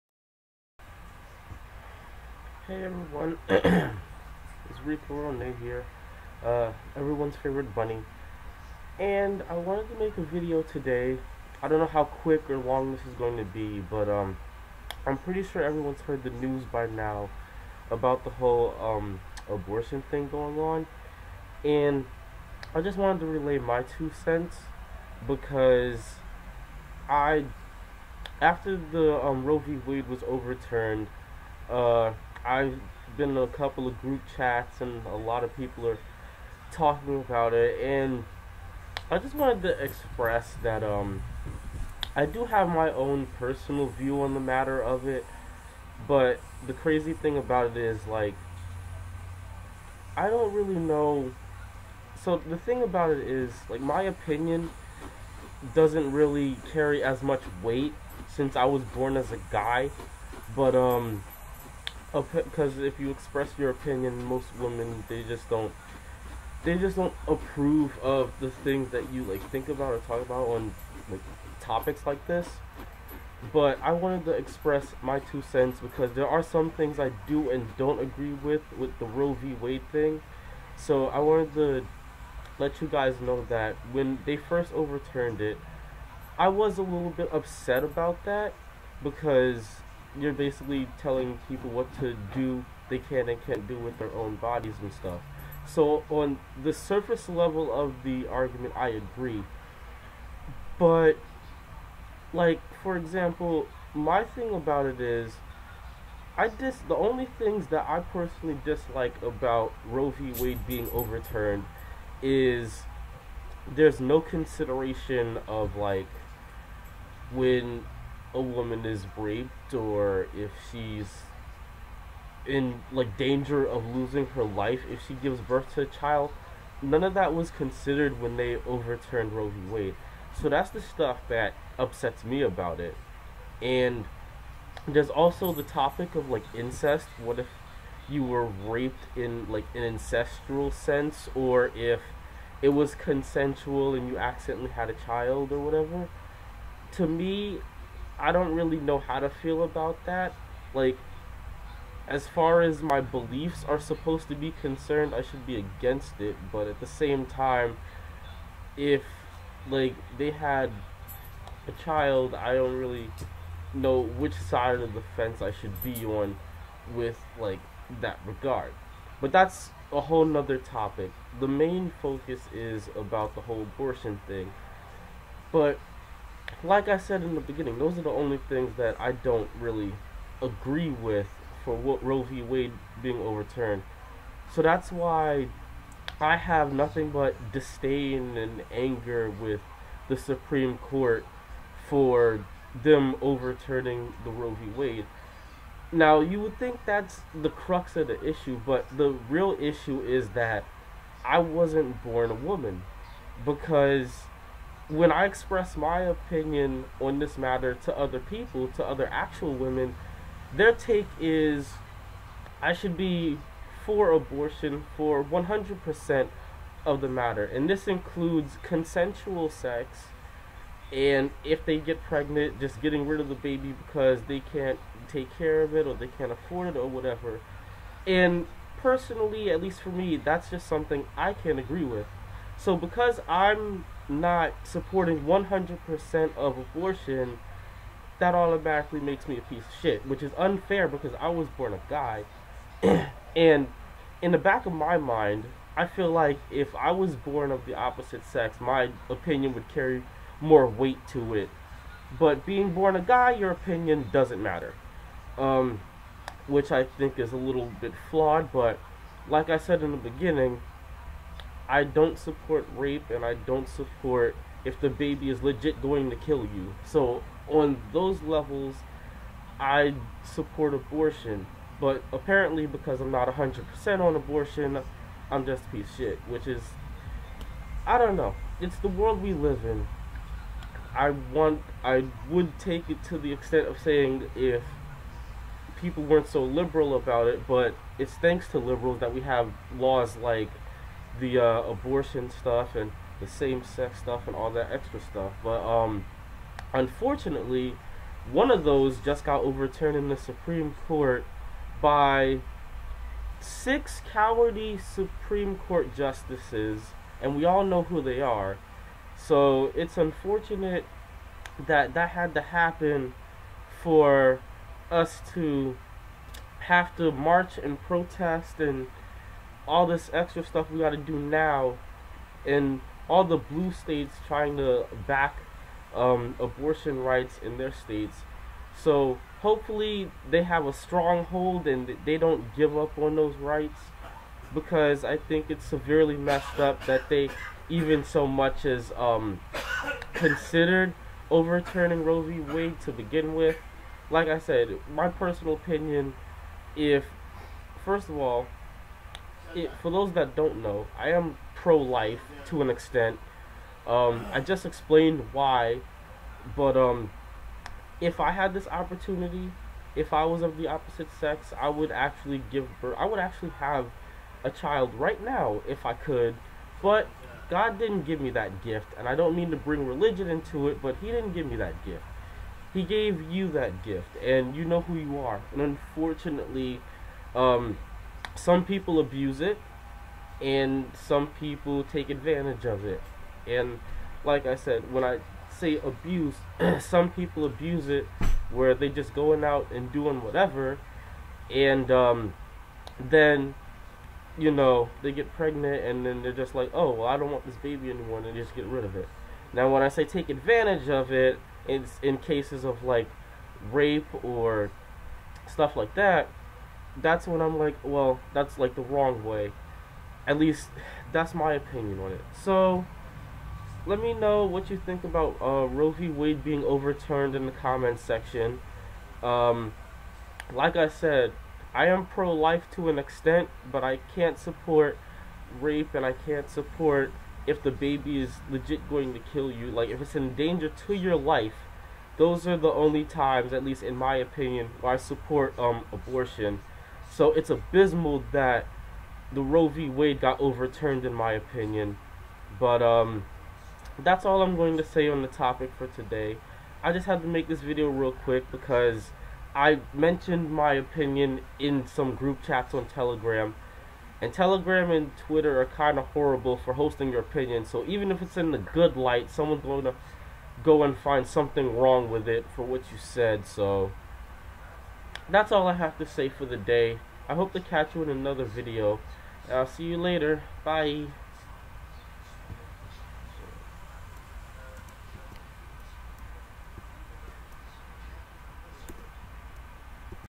My Opinion on Roe v Wade OverTurnment
Long Story Short I think the Roe v Wade Over Turnment was abysmal decision by the current Supreme Court Of The United States (SCOTUS)

-Women shouldn't have to give birth to rape babies
-Women shouldn't have to give birth to babies if they are an endagerment to human life
-(debatebale) Women shouldn't have to give birth to babies born of incest
-Women shouldn't however get rid of babies just because, in cases of consensual sex

Fun Fact, because of that last point I'm automatically a piece of shit -to some people- I would if I could give birth to a baby with concent, but sadly I was born a guy and not a girl. That gift has been given to you women and while I do support my body my choice. I don't believe in just killing off a life, and yes this includes financial situations. I would go so far as to say if you didn't want the baby you should have your legs closed, BUT that would be and is seen as ignorant so I will not use that context. Irreguardless I don't support rape and I can't support being forced to give birth to a child if it's an endangerment to your life as far as incest... I'm not gonna judge so pick your poison with that one, but if there is concent I personally see no reason to abort a child. but that's my opinion, so if you have a diffrent one I'd love to hear your comments down below... and as always thanks for watching these topic are important to talk about